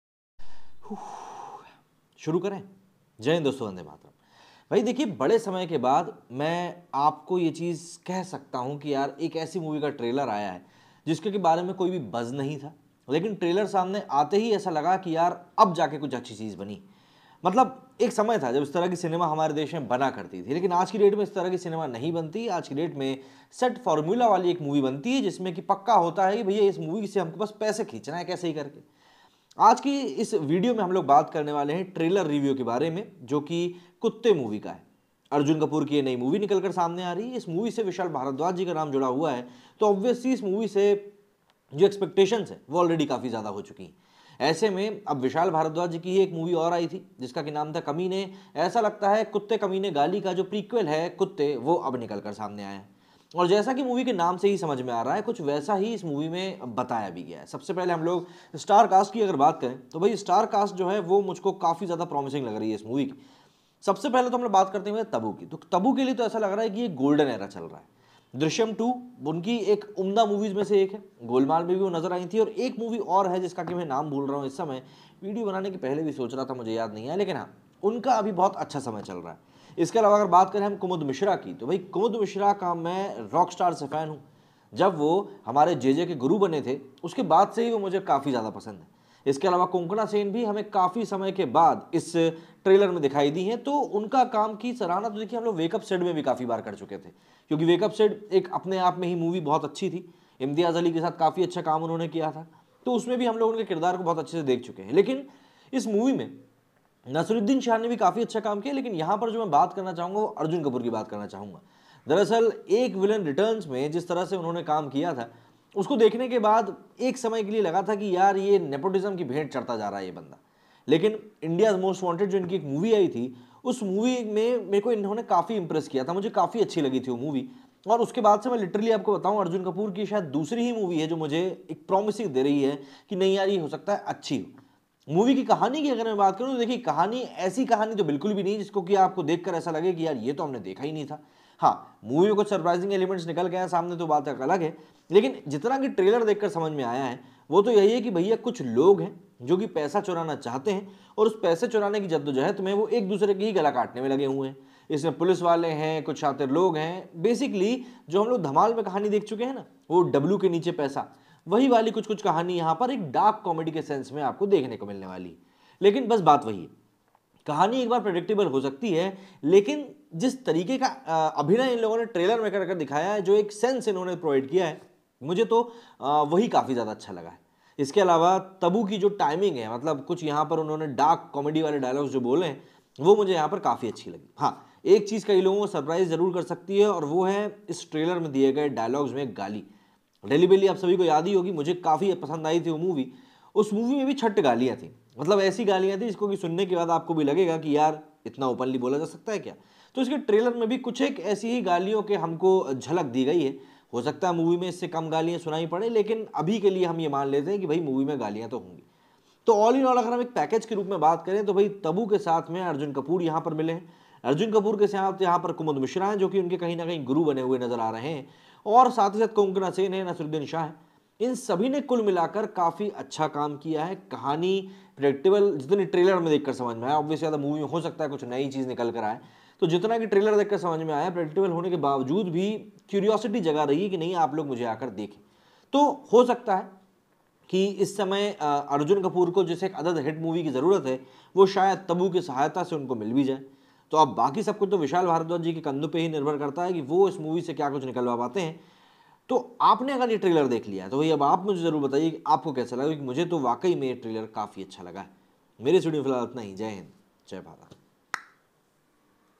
Kuttey Trailer Review: Kaminey भी होंगे? | Arjun Kapoor | Tabu | Naman Sharma
Kuttey is an upcoming Indian Hindi-language 2023 black comedy thriller film directed by Vishal Bhardwaj's son, Aasmaan Bhardwaj, in his directorial debut. It is produced by Vishal Bhardwaj Films, T-Series Films and Luv Films. The film stars an ensemble cast of Arjun Kapoor, Tabu, Naseeruddin Shah, Radhika Madan, Konkona Sen Sharma, Kumud Mishra and Shardul Bhardwaj.

It is scheduled for theatrical release on 13 January 2023.